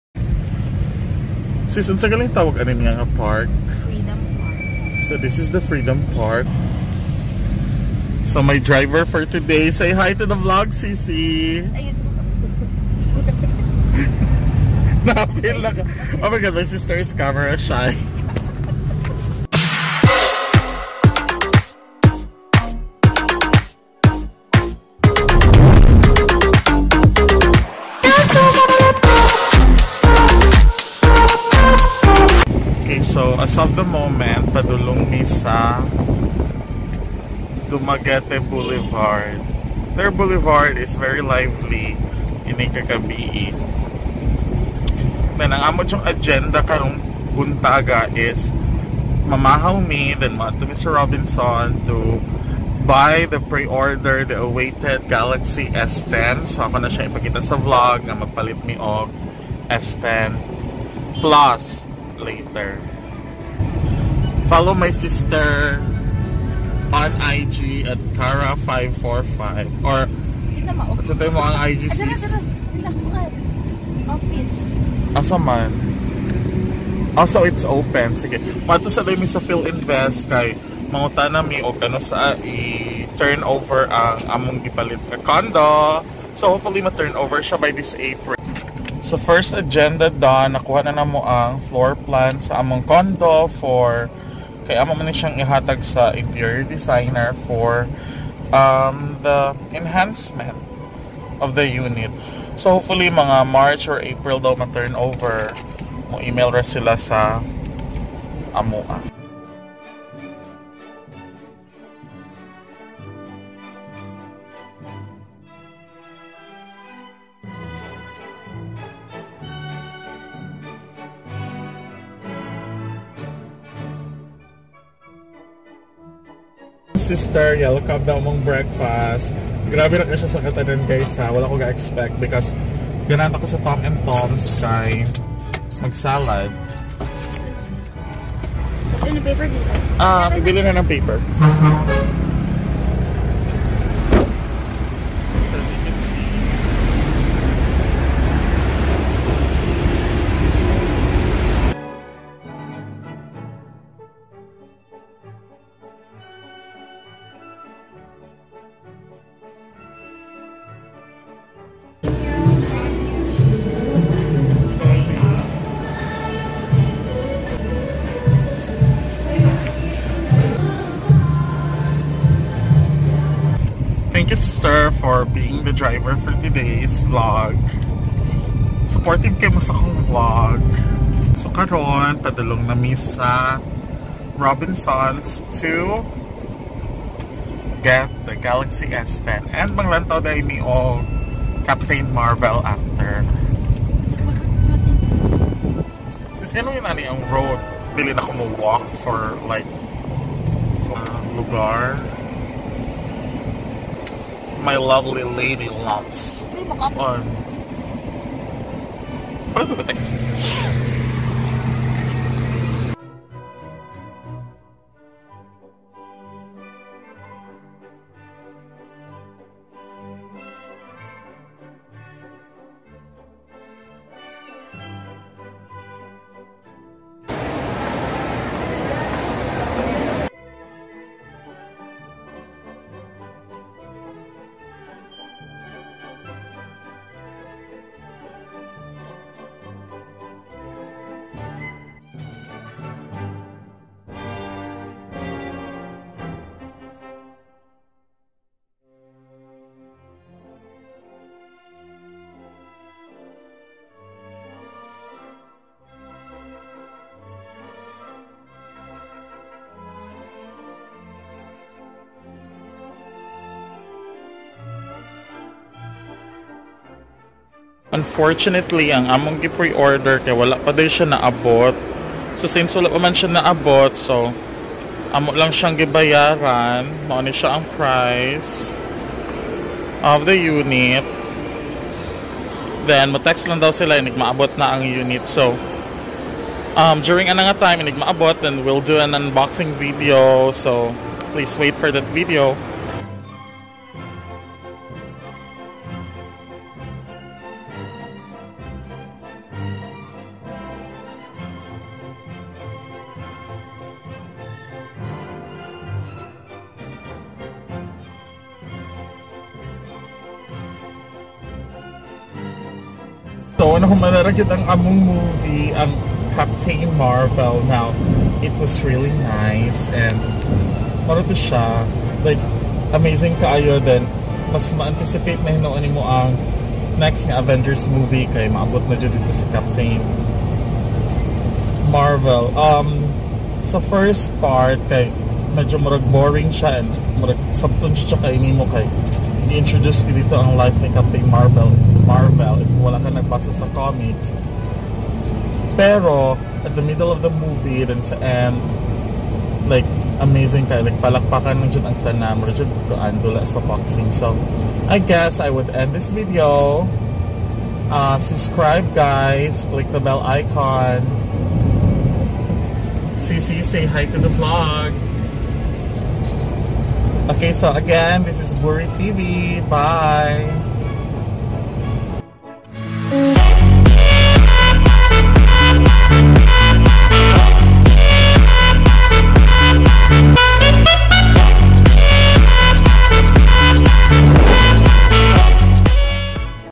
SAMSUNG GALAXY S10+ "Nag preorder, binili, na-disappoint"
Dumaguete City Vlog 1 and buying the newest and greatest of samsung , SAMSUNG GALAXY S10+